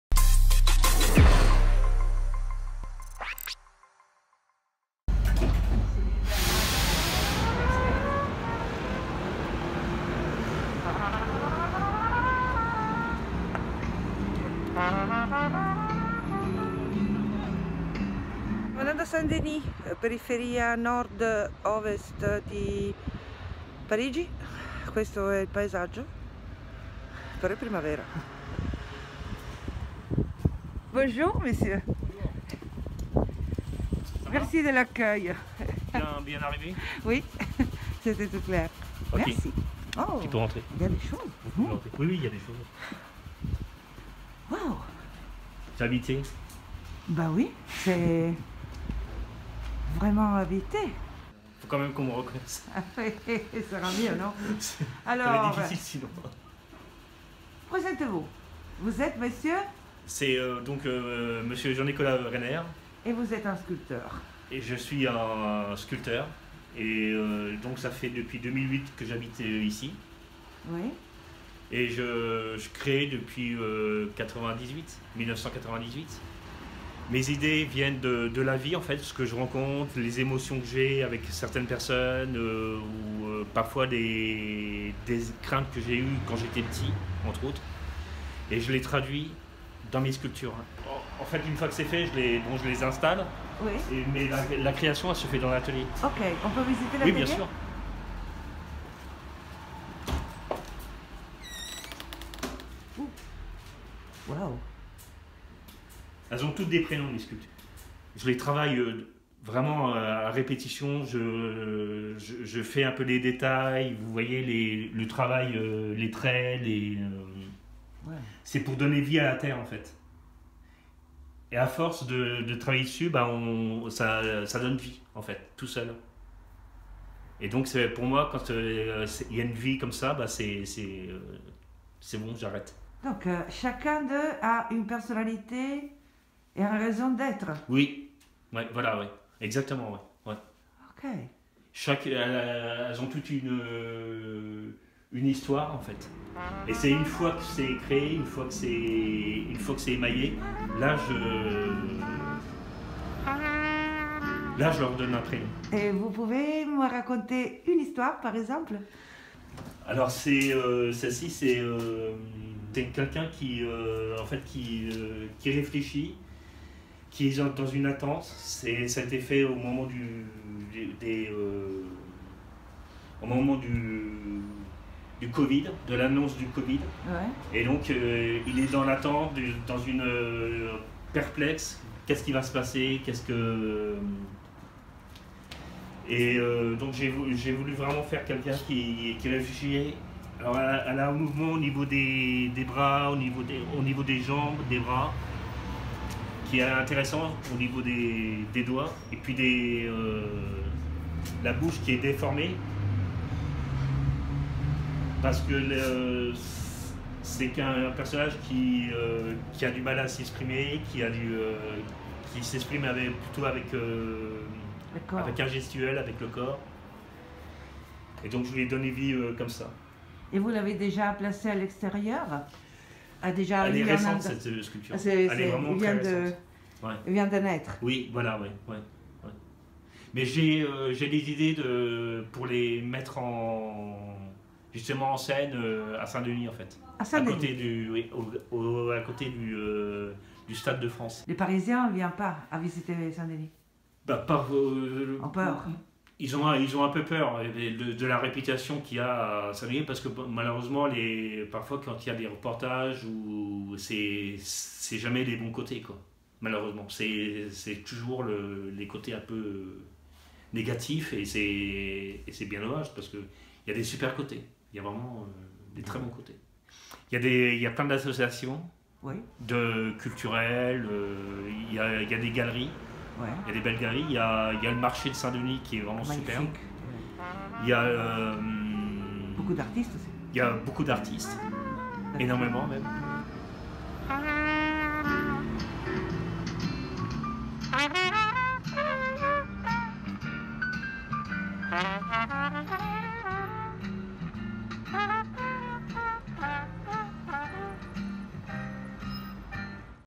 ❯❯ 𝗜𝗣𝗘𝗥 | Grand Parì - Nicolas Reinert "Les Personnages"
Grand Parì è a cura di Simona Amelotti / Metacritikopera

Jean Nicolas Reinert scultore (art brut) nato nel 1968 à St Quentin, vive e lavora a Saint-Denis, periferia nord- nord ovest di Parigi.

Una visita all’atelier dell’artista con una breve intervista sulla creazione di sculture che “vivono” solo in relazione allo spazio urbano. Attraverso le fotografie, montate nel film, l’artista espone gli stati d’animo che l’argilla assorbe durante la lavorazione. Questa pratica gli permette di liberarsi dei fardelli legati ai traumi pregressi inscritti nella storia dello sviluppo urbano della periferia di Parigi.

IPER Festival delle periferie è realizzato da Museo delle periferie
promosso da Roma Culture, Azienda Speciale Palaexpo.